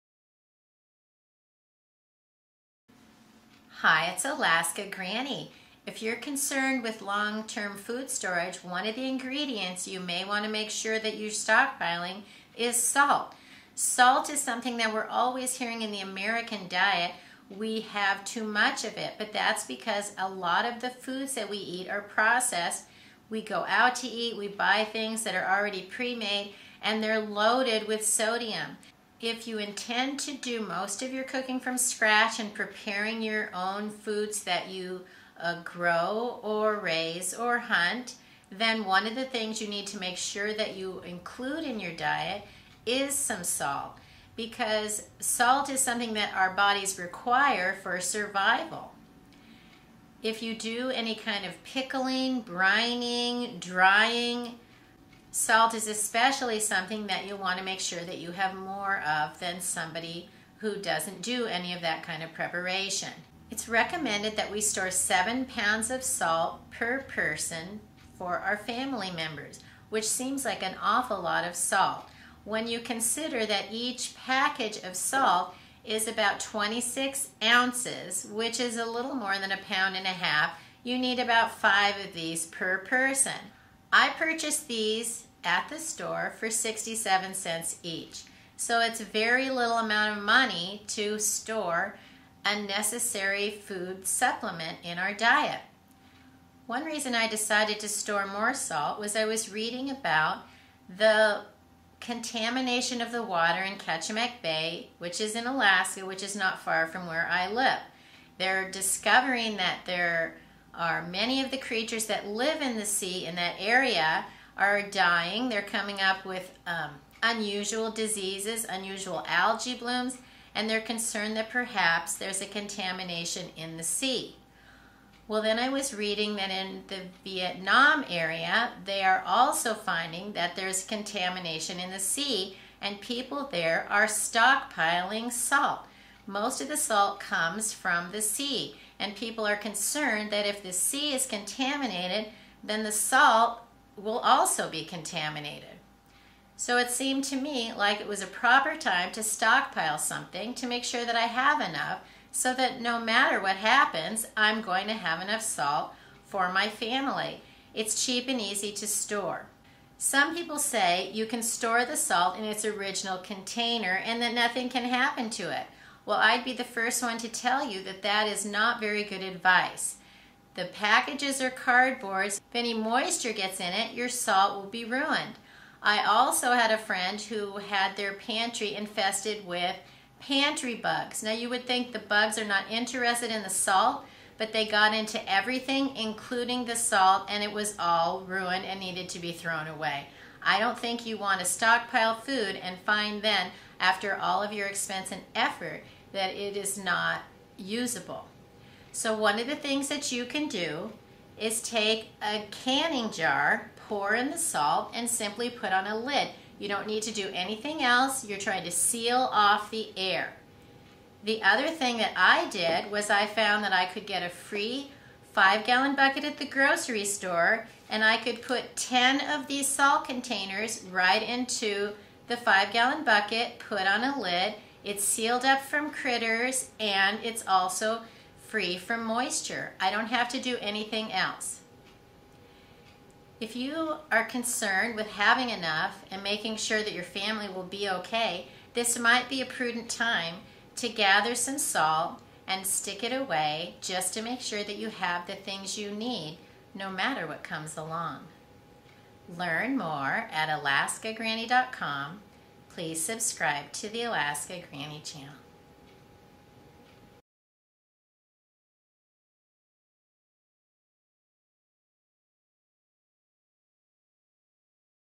How and Why to Store Salt Long Term Food Storage
How and Why to Store Salt Long Term Food Storage Salt is an important food to include in your prepper pantry long term food storage.  Salt How to store salt right, so it will last with your prepping supplies?  Salt is necessary for life, a long lasting food that can last indefinitely if stored correctly.  Salt is a crucial prepping item and needs to be kept dry, the cardboard container may not be ideal.  Store salt in canning jars, and screw on a lid.  Do not use oxygen absorbers.  Salt boxes can also be stacked inside 5 gallon buckets.  Keep moisture and pests from contaminating your foods stored for long term.  Store salt in containers that allow ease of use.  Full buckets of salt are not as convenient as a jar or original box. Are you a prepper, prepping and stockpiling?  Stocking Up On Food?  Recommendations are to store 7 pounds of salt per person.  If you use salt for pickling, brining, and preserving, you may need to stockpile more.  At less than a dollar a pound, salt is cheap and easy to stockpile and store.   Salt can be used for first aid, hygiene, cleaning, and even pest control, and may be one of the most important prepping supplies.  Indications are that some areas of the sea are becoming contaminated.  Most salt comes from the sea.  You don't have to be a Prepper to stockpile food.  It's just sensible. Stockpile salt for your family while it is still safe and readily available so you will always have the things you need, no matter what comes your way.

★☆★ RECOMMENDED RESOURCES: ★☆★
Gamma Lids
Canning Jars
Plastic Jar Lids
5 Gallon Buckets and Lids